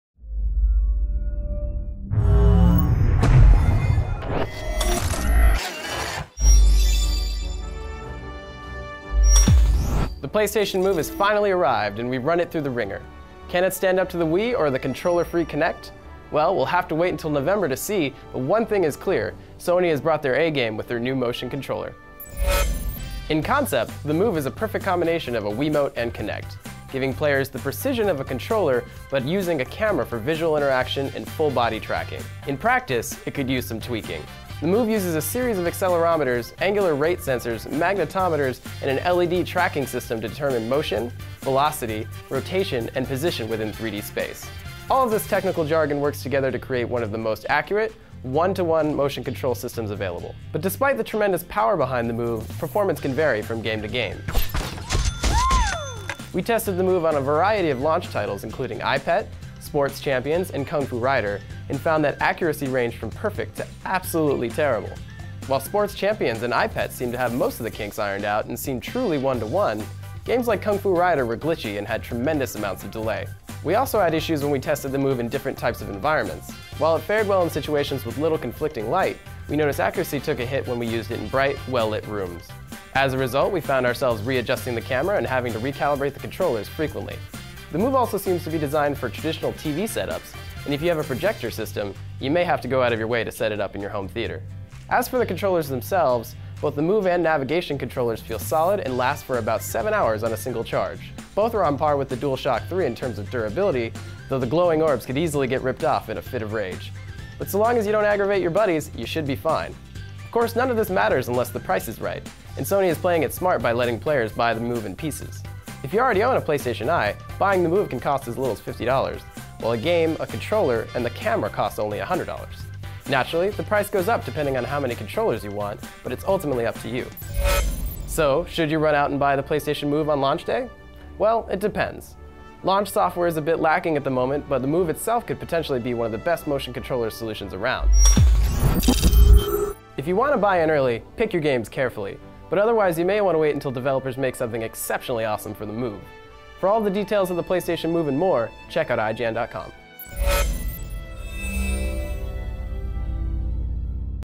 PlayStation Move Review
PlayStation Move is Sony's answer to motion-enabled video gaming. IGN puts the hardware to the test and reviews it. Watch and see if it's worth your time and dime.